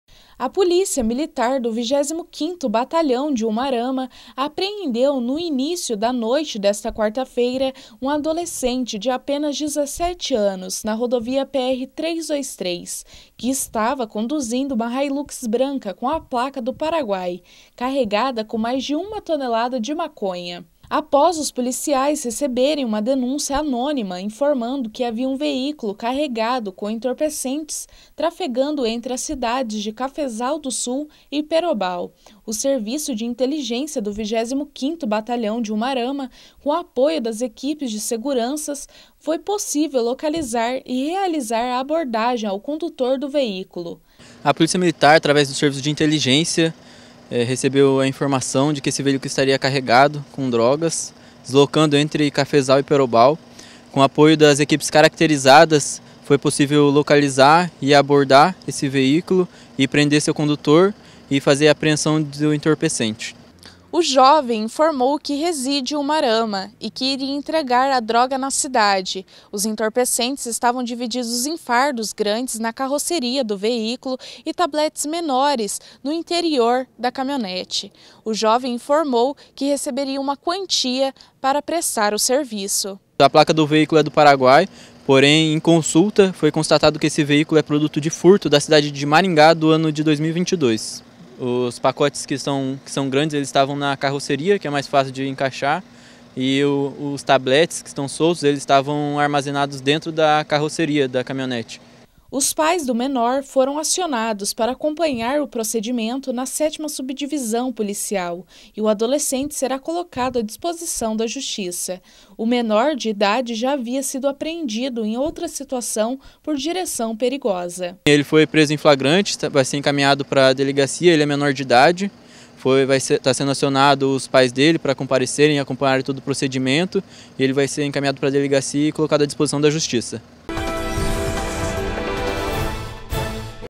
Adolescente é apreendido com mais de uma tonelada de maconha na PR-323
No total, foram apreendidos 1.013 quilos de maconha e R$ 1.042,00 em dinheiro, um prejuízo ao crime organizado de R$ 1 milhão.